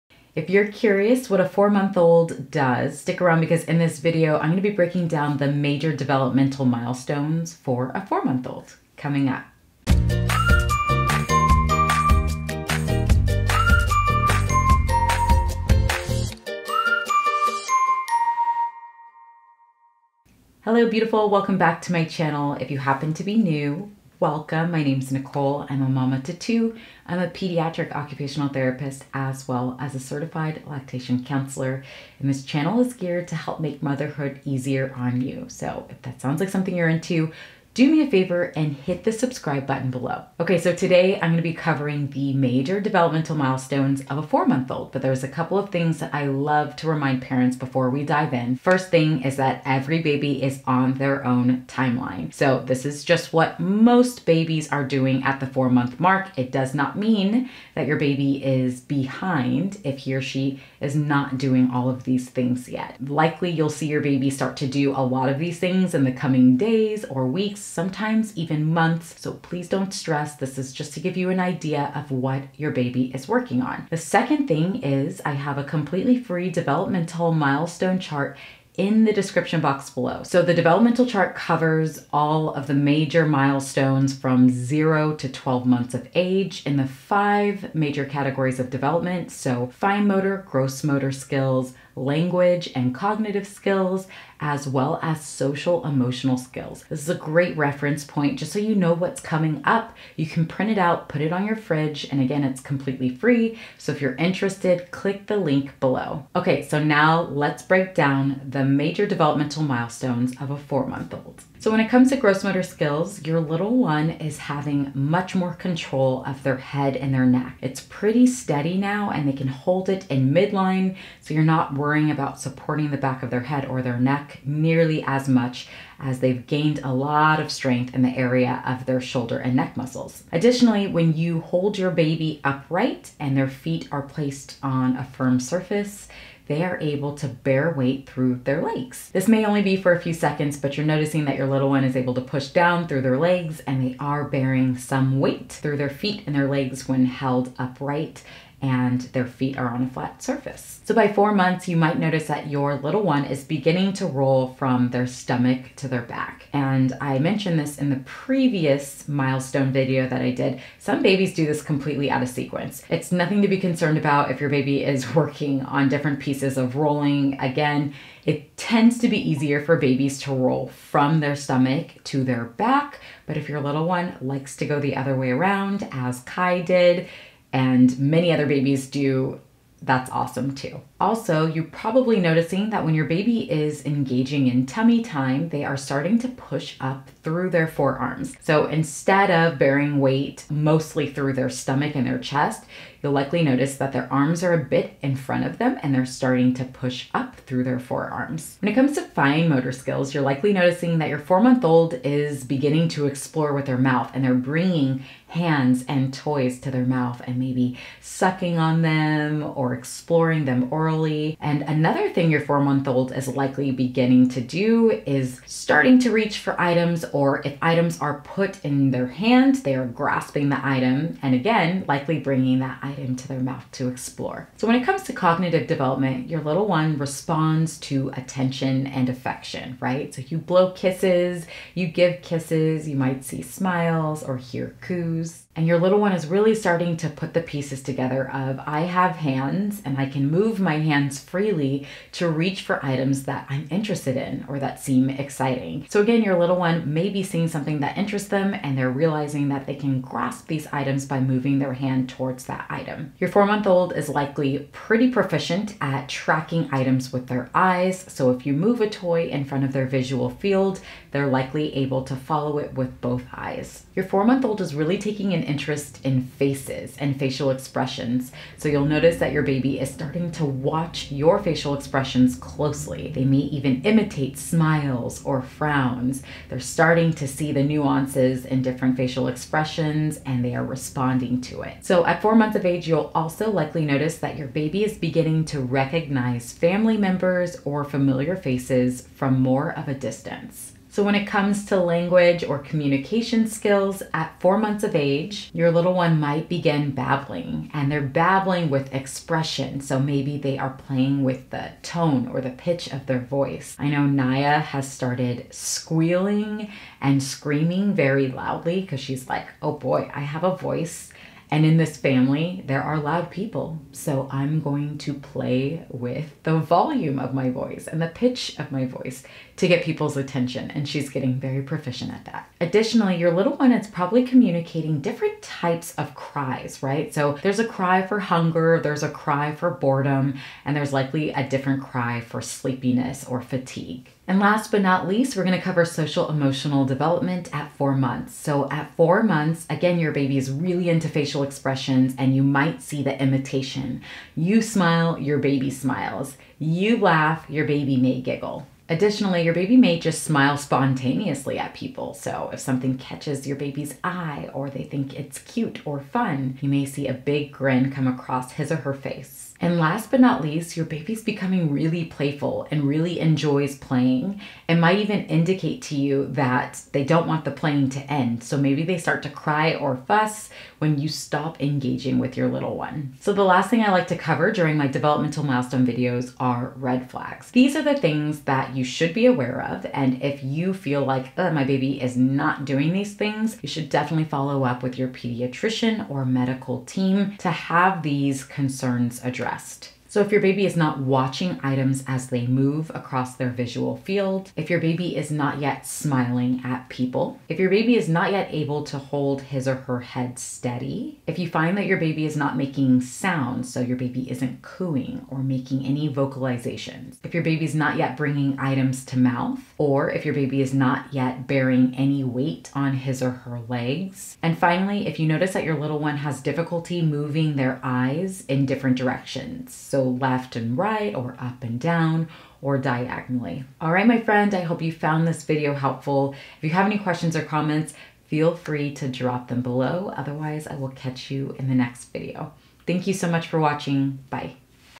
4 MONTH OLD BABY DEVELOPMENTAL MILESTONES: Cognitive, Social-Emotional, Language, Gross & Fine Motor
4 MONTH OLD BABY DEVELOPMENTAL MILESTONES: Fine Motor Skills, Gross Motor Skills, Language Development, Social-Emotional Skills, And Cognitive Skills for your 4 MONTH OLD BABY.

Hi, Moms and Dads! In today's video, I am going over the 4 month old baby development categories that you should expect to see your 4 month old reaching soon, if they haven't already. I discuss and give tips for the five main areas your little one is developing in; Fine Motor Skills, Gross Motor Skills, Language Development, Social-Emotional Skills, and Cognitive Skills.

Chapters:
0:00 Intro
1:53 4 Month Old Developmental Milestones- Gross Motor
3:20 4 Month Old Development Fine Motor Skills
3:45 4 Month Old Cognitive Development
4:50 4 Month Old Language And Communication
5:36 4 Month Old Social-Emotional Development
6:18 4 Month Old Development Red Flags